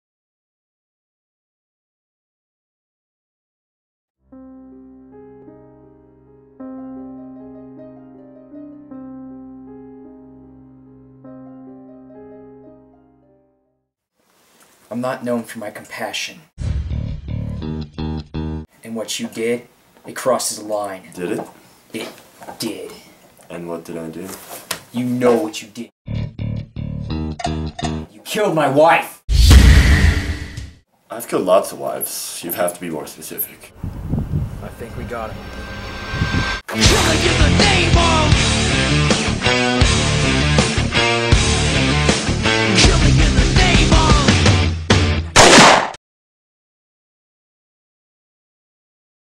Killing in the Name - TRAILER
Trailer for a 5 min. film we made in Movie Club
Directed by: Tyler P
Screenplay by: Tyler P and Sam B
Starring: Branson N
Austin MC
Bridgit H
Sam B